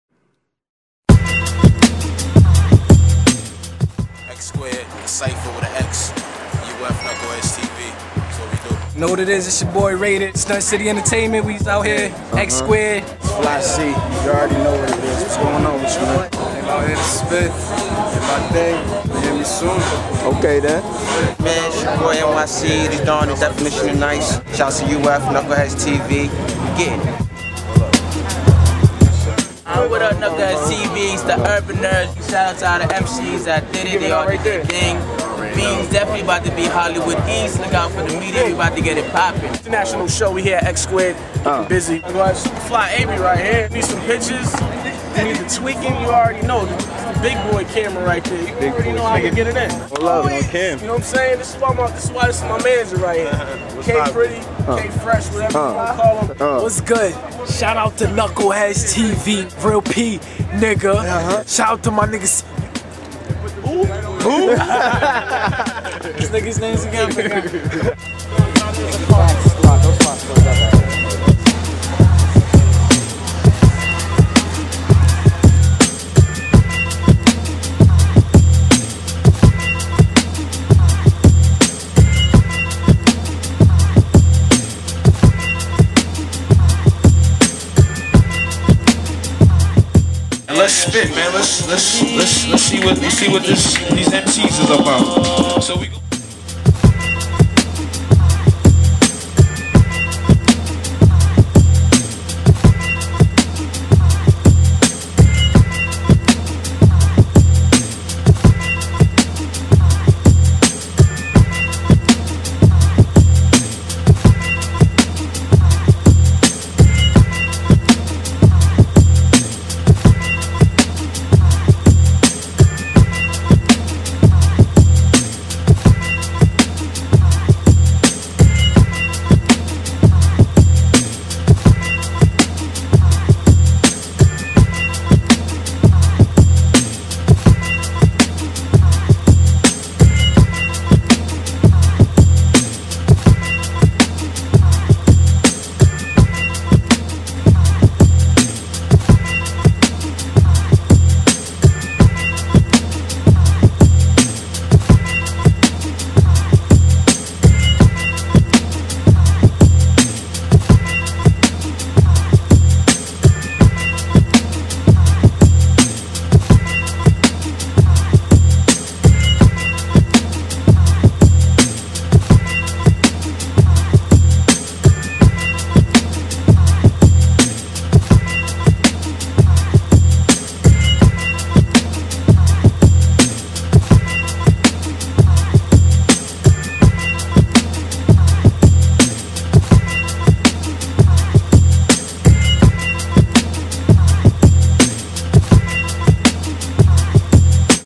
The CXPHER Preview
A preview to a historic event that took place @ X-Squared on Newbury Street. A handful of the premiere rap artists from around Boston came together to take part in The CXPHER!